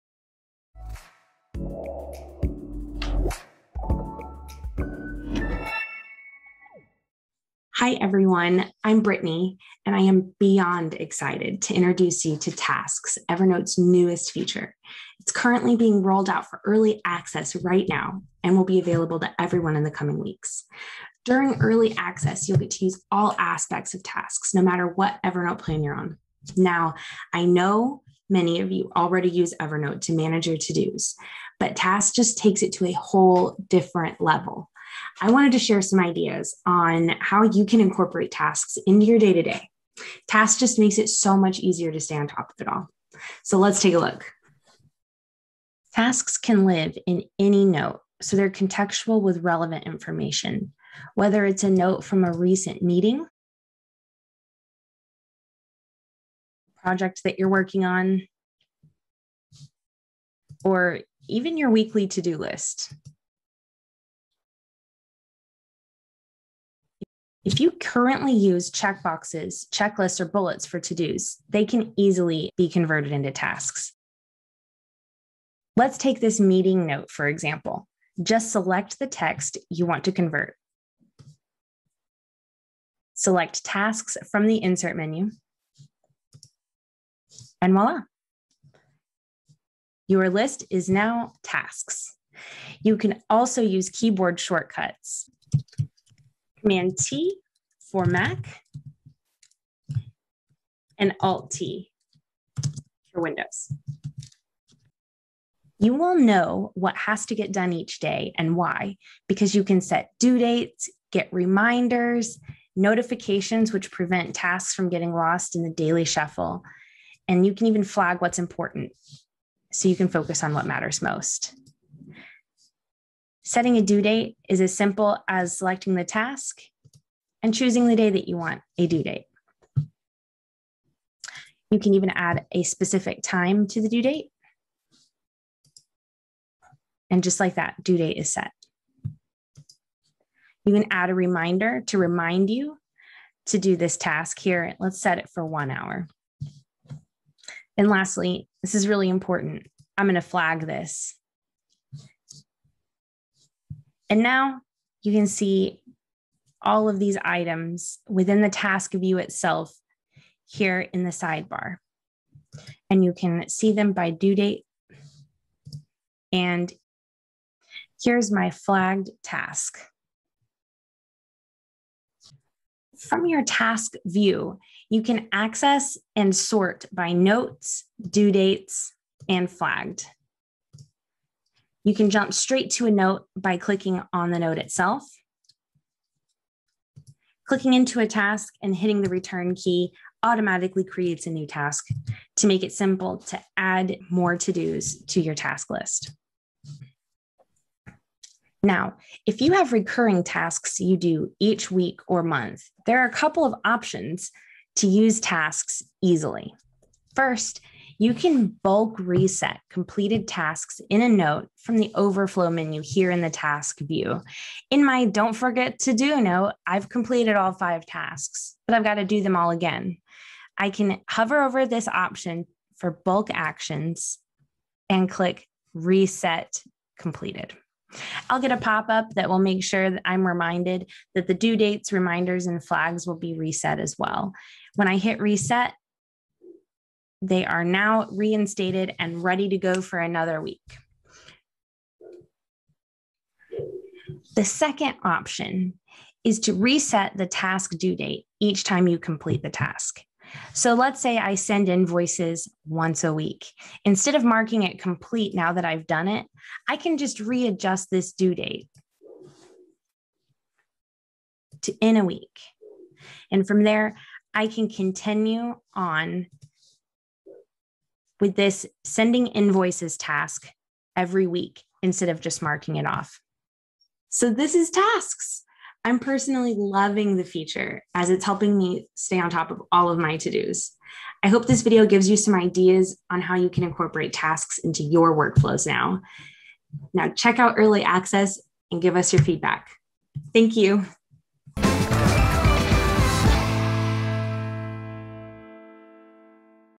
5 ways to start using Tasks in Evernote TODAY!
If you have been using Evernote for a while, this video will help show you how to easily start using Tasks in your existing workflows. In this video, Brittany will show you 5 ways to begin to integrate Tasks easily into Evernote.

1. Tasks can be note specific and exist alongside contextual information in your notes, such as tasks for a project or meeting.

2. Existing lists such as bulleted lists, checklists, checkboxes, and even lines of text can be easily converted to a Task in Evernote with a single click of the Task option in the Insert Menu.

3. Unlike the way lists work in Evernote, Tasks can have individual due dates, reminders, and important flags. So now each to-do can be managed differently, even if you have multiple to-do's in a single note.

4. If you have to-do's across many different notes, converting them to Tasks will now allow you to see ALL your tasks in one place in the Task View. With many different filter options in the Task View, you can sort by flagged, due date, completed, and much more.

5. There are also a few different ways to manage recurring to-do's using Tasks now, such as bulk resetting completed items, or simply shifting due dates.

There is so much to learn and do with Tasks in Evernote! For more information on how to manage and organize your Tasks in Evernote, tune into our YouTube channel